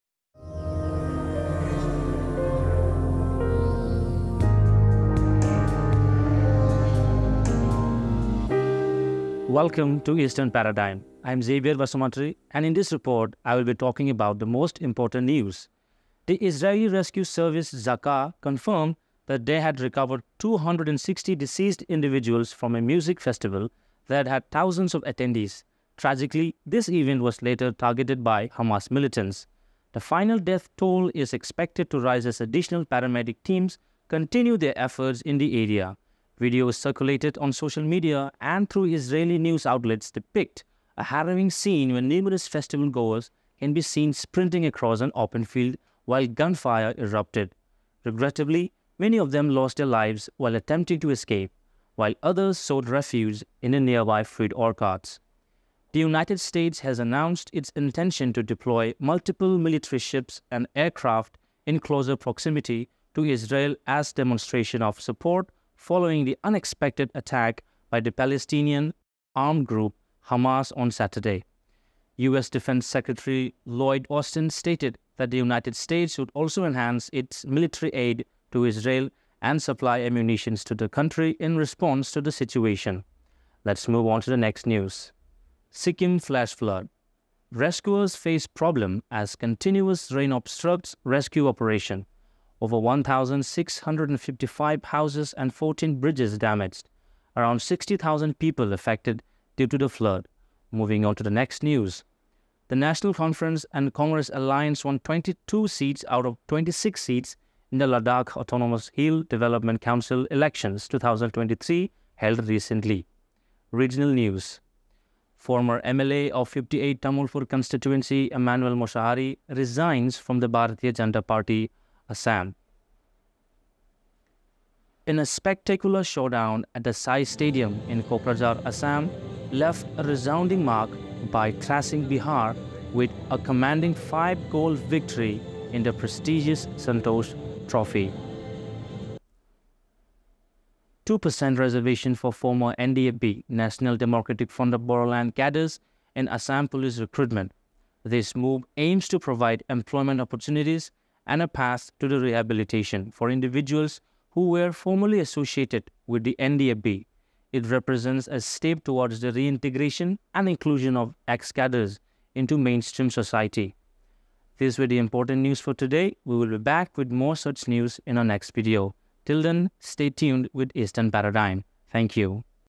World News || Israel - Palestine War Day 3 || Sikkim flash-flood || Santosh Trophy
Israel updates
On Sunday, October 8th, the Israeli rescue service Zaka confirmed that they had recovered 260 deceased individuals from a music festival that had thousands of attendees. Tragically, this event was later targeted by Hamas militants.
The final death toll is expected to rise as additional paramedic teams continue their efforts in the area. Videos circulated on social media and through Israeli news outlets depict a harrowing scene where numerous festival-goers can be seen sprinting across an open field while gunfire erupted. Regrettably, many of them lost their lives while attempting to escape, while others sought refuge in nearby fruit orchards.

The United States has announced its intention to deploy multiple military ships and aircraft in closer proximity to Israel as a demonstration of support following the unexpected attack by the Palestinian armed group Hamas on Saturday. On Sunday, US Defense Secretary Lloyd Austin stated that the United States would also enhance its military aid to Israel and supply munitions to the country in response to the situation.
Rescuers face problem as continuous rain obstructs rescue operation. Over 1,655 houses and 14 bridges damaged. Over 60,000 people affected due to the flood.
The national conference and congress alliance won 22 seats out of 26 seats in the Ladakh autonomous hill development council.
Former MLA of 58th tamalpur constituency Immaneul moshahary resigns from the Bharatiya Janata party, Assam.

In a spectacular showdown at the SAI Stadium in Kokrajhar, Assam left a resounding mark by thrashing Bihar with a commanding 5-goal victory in the prestigious Santosh Trophy.
2% reservation for former NDFB (National Democratic Front of Bodoland) cadres in Assam Police recruitment. This move aims to provide employment opportunities and a path to rehabilitation for individuals who were formerly associated with the NDFB. It represents a step towards the reintegration and inclusion of ex-cadres into mainstream society.